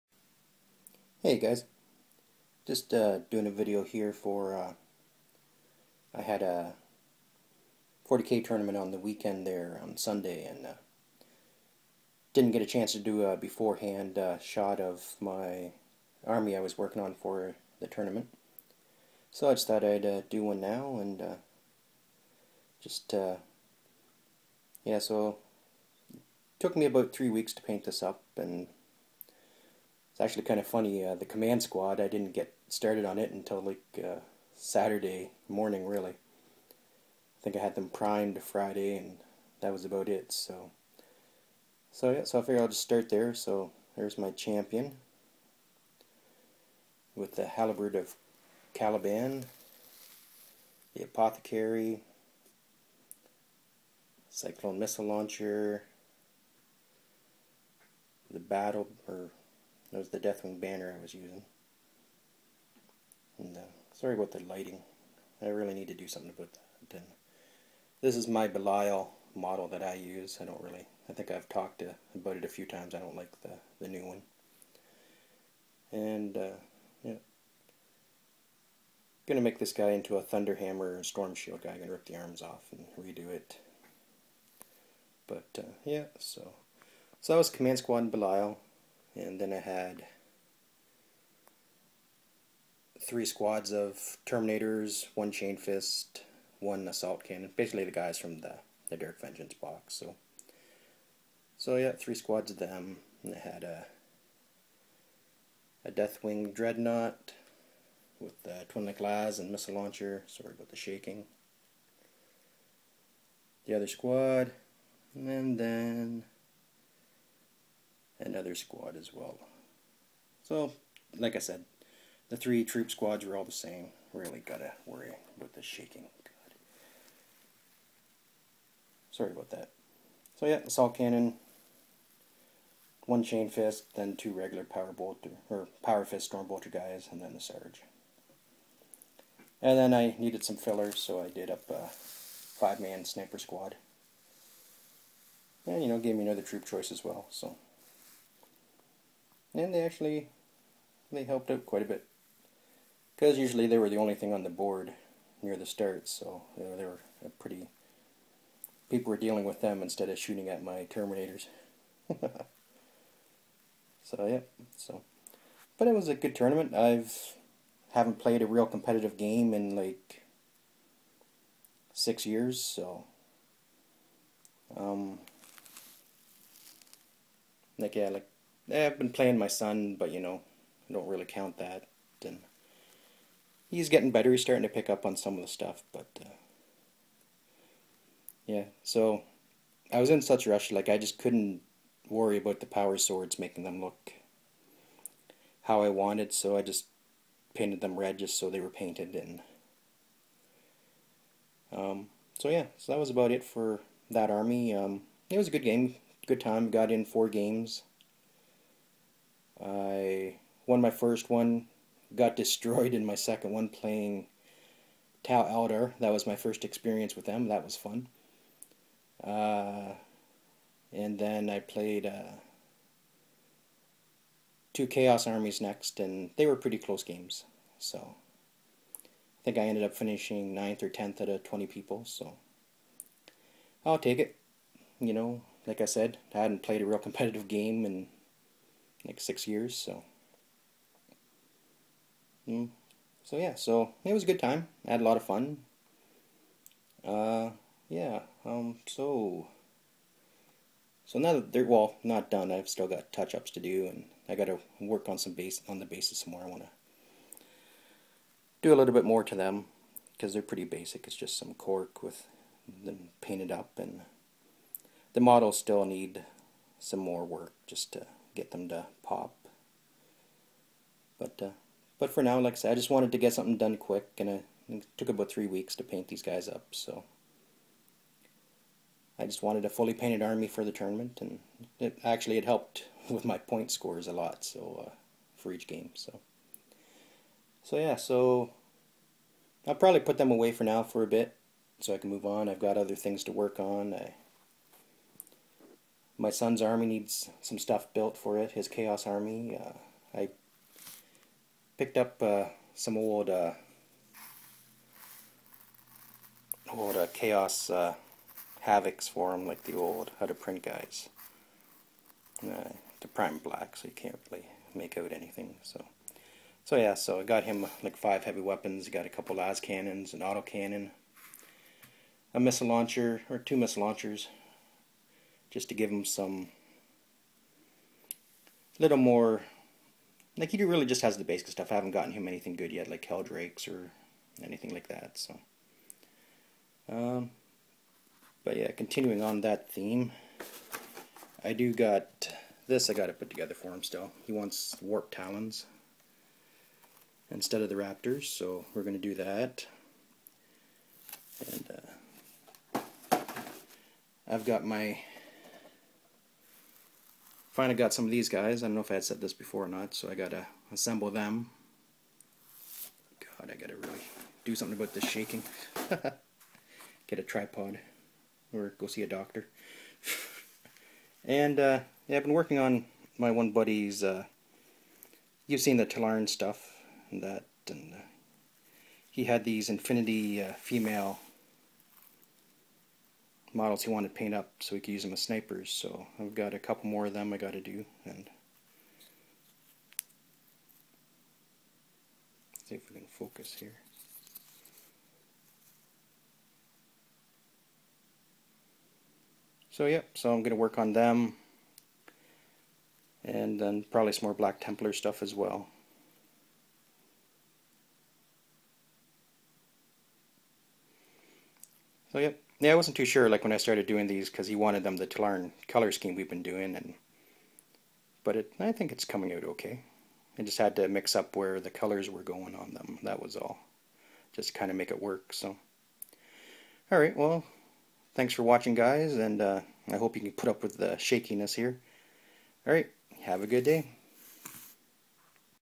Deathwing army and other projects
finsihed deathwing army for the tournament and other wip items on the go